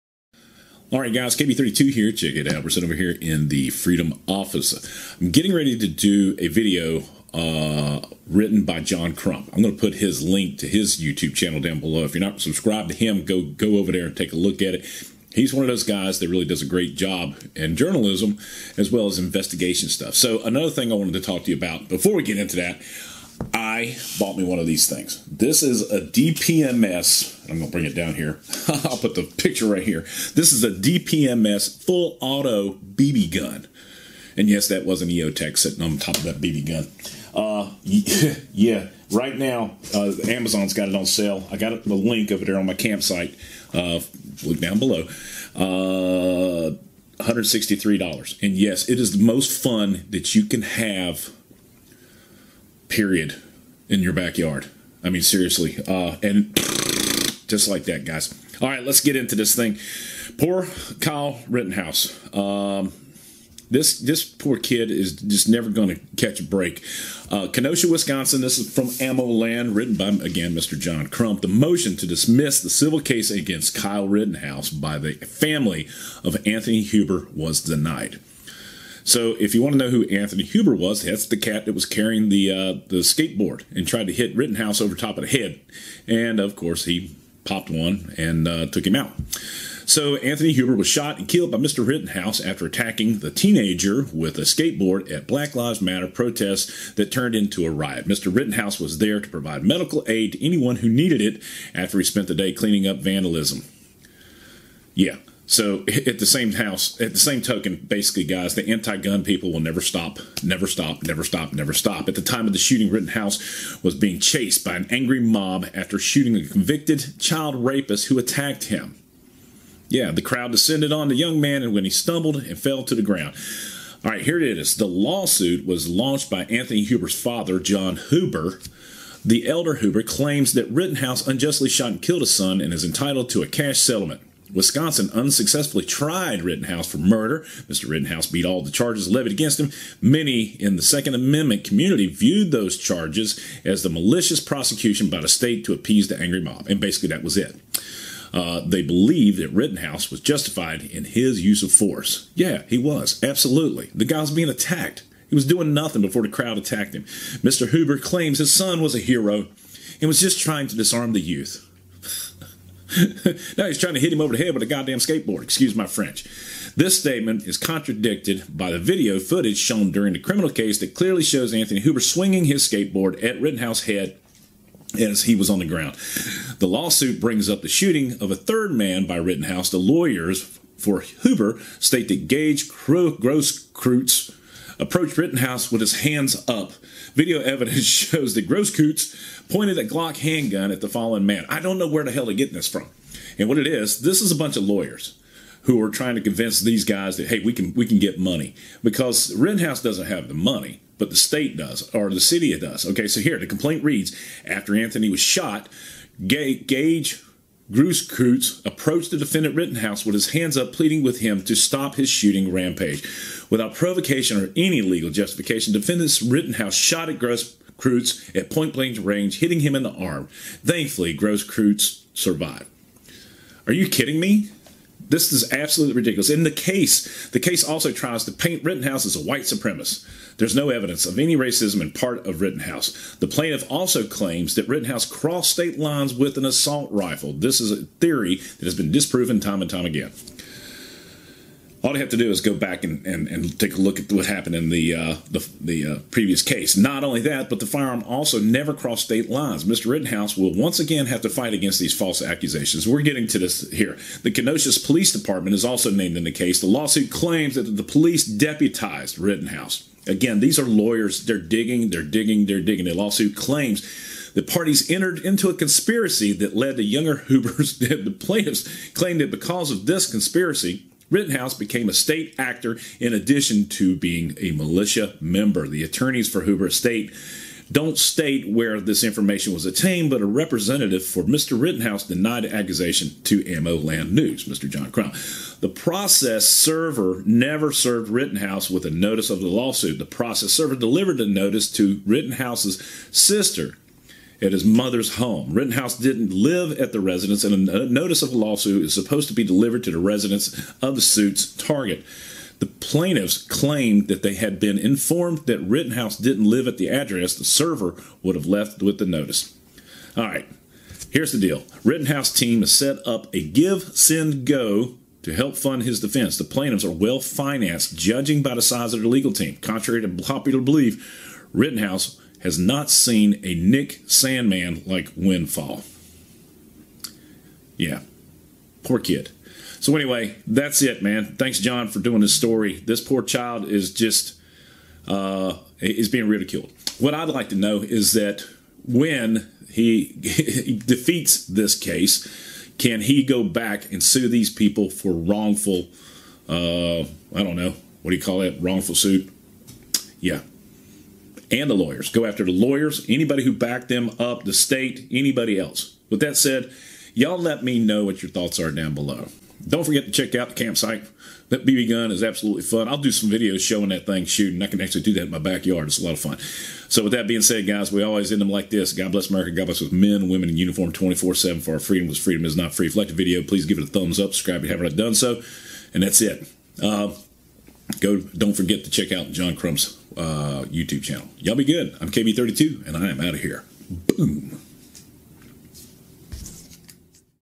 Court Denies to Dismiss Civil Suit Against Kyle Rittenhouse
Poor Kyle Rittenhouse is never going to be left alone.  The Mob will chase him forever.